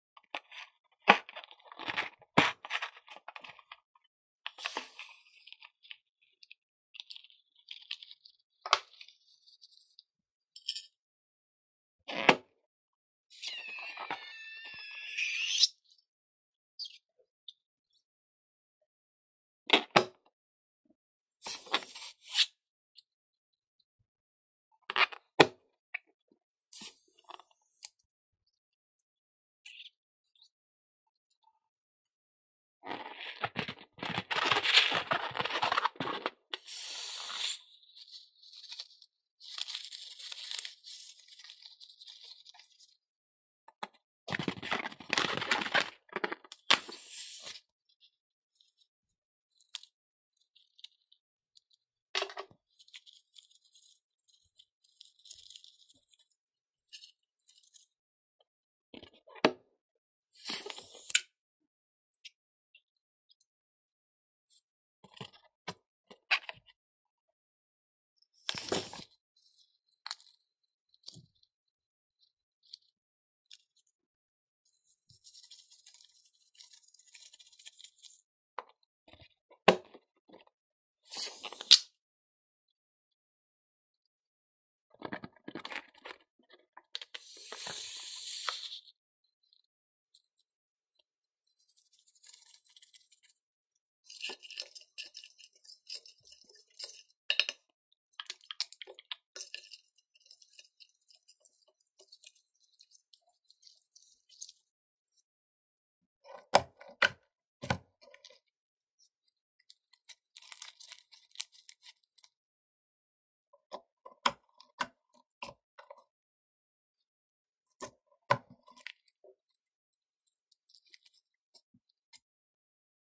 Relaxing Making Slime with Funny Balloons ! ASMR ! Part 333
Hey guys! Today I'm going to be Making Slime With my new Funny Balloons. Hope you enjoy this Relaxing Satisfying ASMR video.